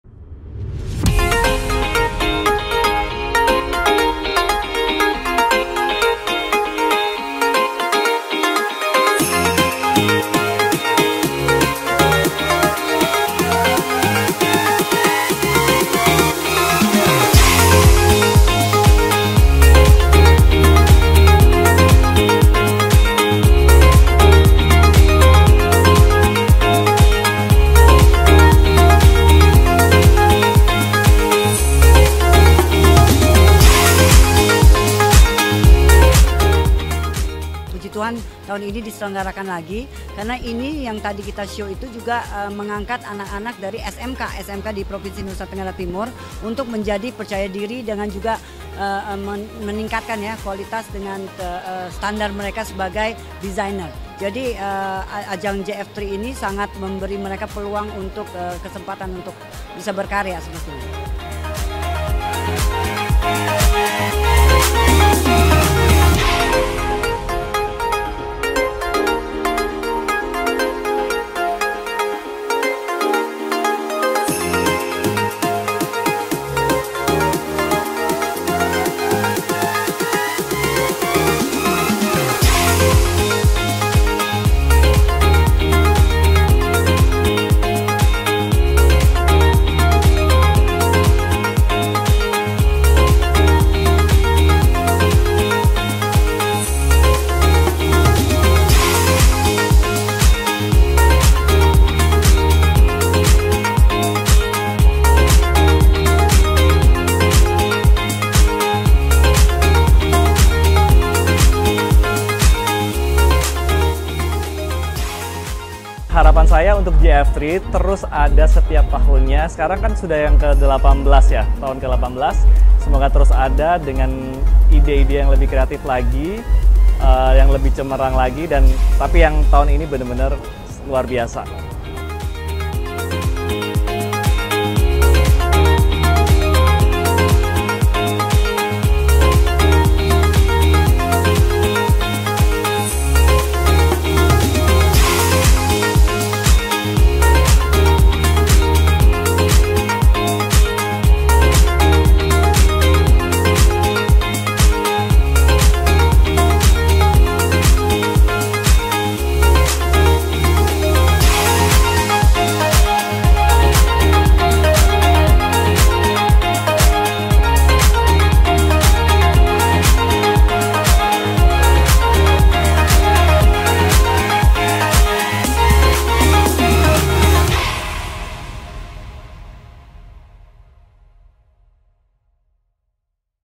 DAY 2 - DEKRANASDA NTT presents La Casa De La Nusa - JF3 Fashion Festival 2022
Throwback Day-2 of JF3 Fashion Festival 2022 ✨

Dekranasda NTT mempersembahkan presentasi koleksi kain Tenun yang bertemakan “La Casa De La Nusa” pada JF3 Fashion Festival 2022 oleh desainer kebanggaan Tanah Air.

Eggie Jasmine
Defrico Audy
Maya Ratih
Temma Prasetio